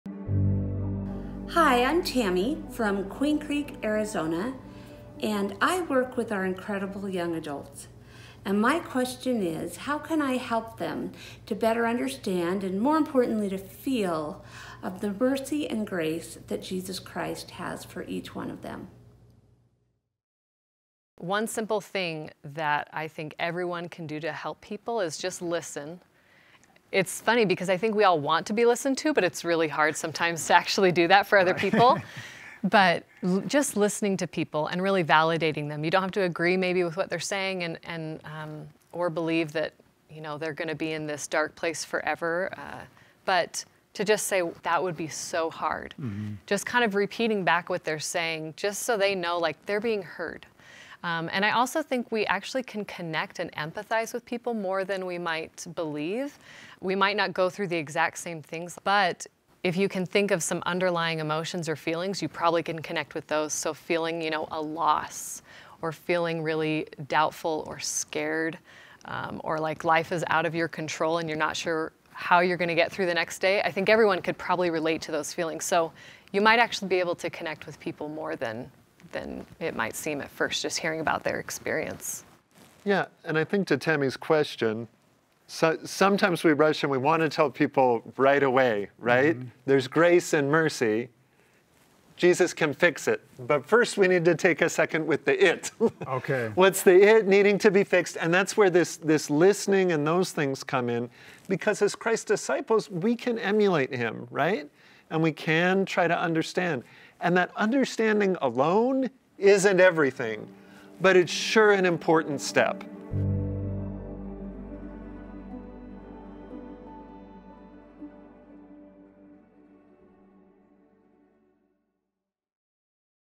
Helping Others Feel Mercy
The panel responds to a viewer question about helping others to find Christ’s grace.